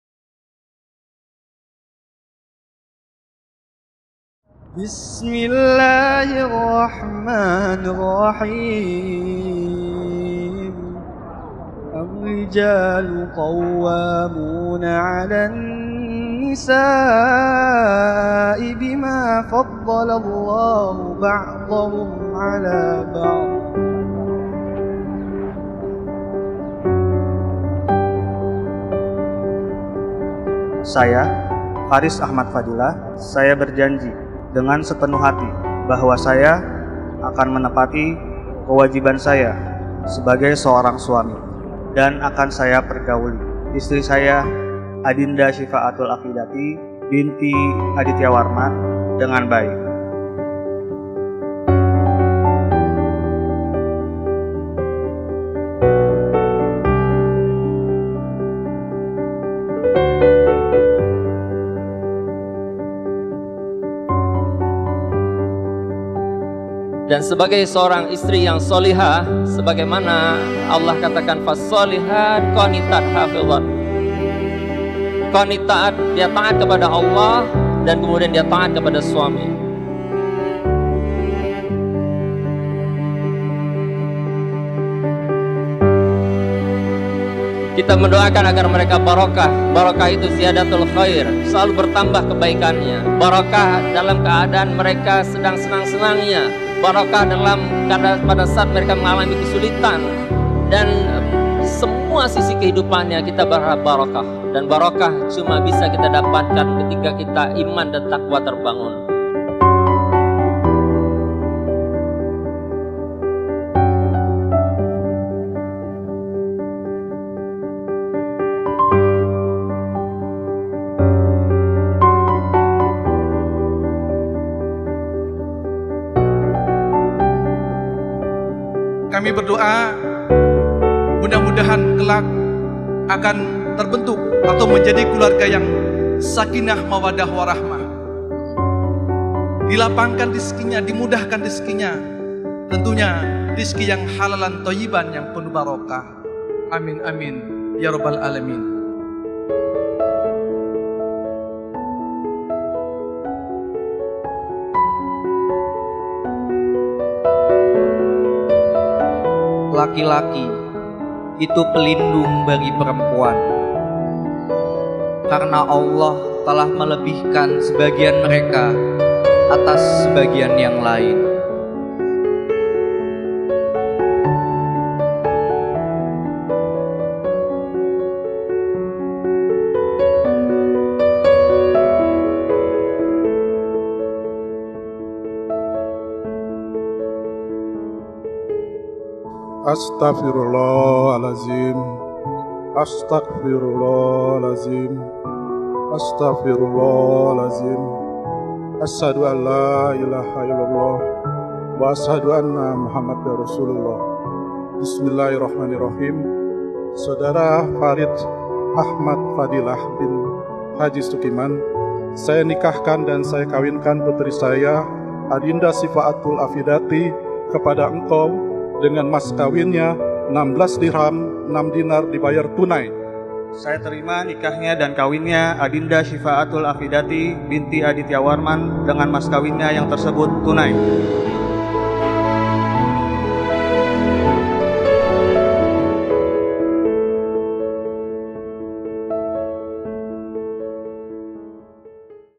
Aliy Photography - Masjid At Tin Wedding Clip Cinematic Highlights - Alt Ver
Masjid At Tin Wedding Cinematic
Courtesy Dinda & Fariz Wedding

Collaboration Video with
99 Motion
Quon Pictura

with love,
Aliy Photography